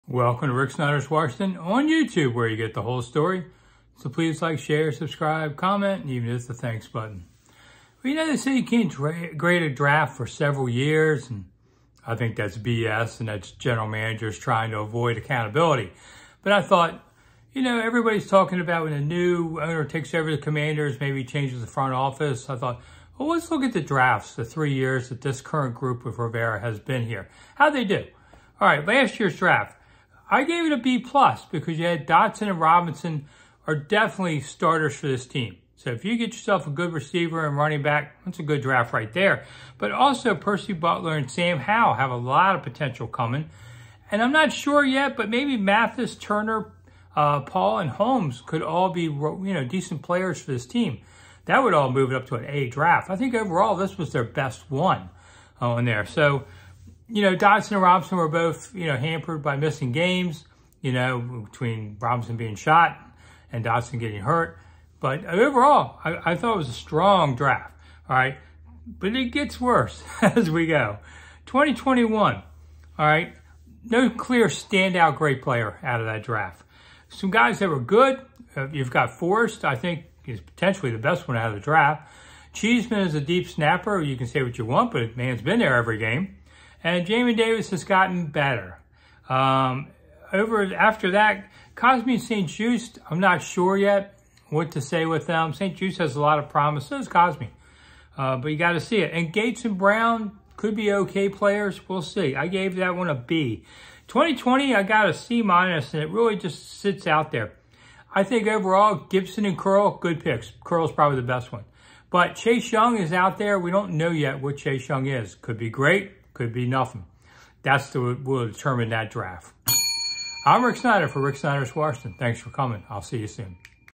Grading last 3 Washington Commanders drafts
Rick Snider’s Washington grades the Washington Commanders last three drafts. Gimme two minutes.